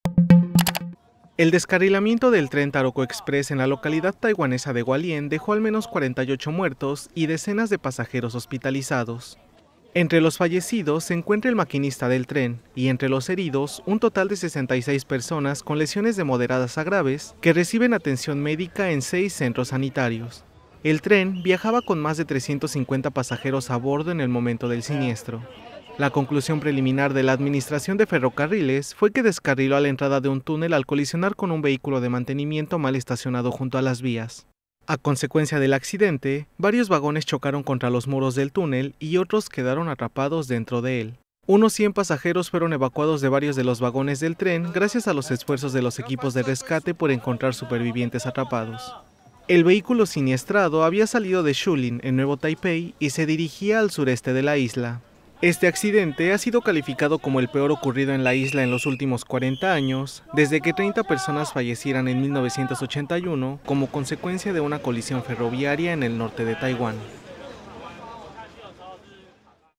Un accidente de tren en Taiwán deja al menos 48 muertos y decenas de heridos
El descarrilamiento del tren "Taroko Express" en las inmediaciones de la localidad oriental taiwanesa de Hualien dejó hoy al menos 48 fallecidos y decenas de pasajeros hospitalizados con heridas de diversa consideración.

¡Visítenos!